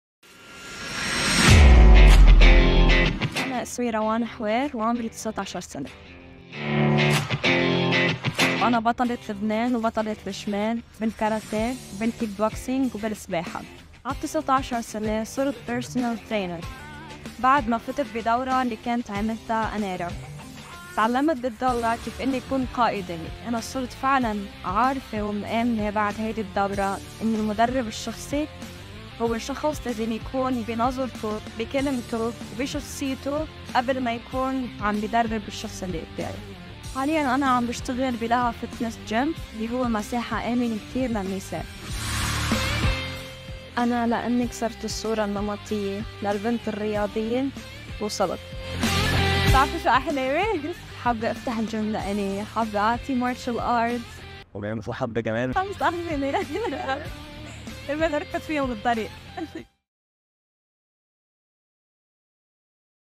Meet Rawan, A Personal Trainer Breaking Down Stereotypes in Lebanon
Rawan hopes to fulfill her many dreams with the knowledge and training she received in Anera's personal trainer course. Personal trainers and physical health practitioners are growing careers amid Lebanon's economic crisis.